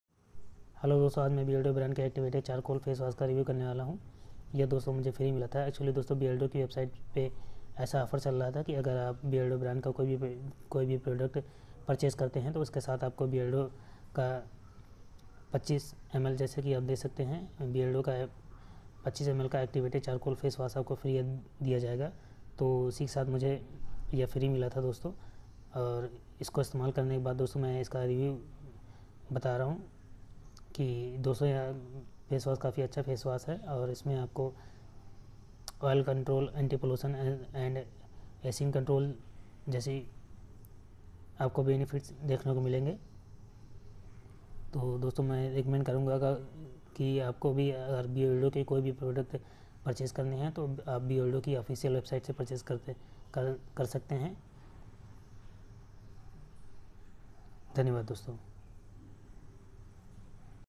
Beardo Activated Charcoal Anti pollution for deep pore cleaning (25 ml ) Review
* Suitable for Acene prone skin
* Removes Dirt and impurities
* Anti pollution
* Oil Control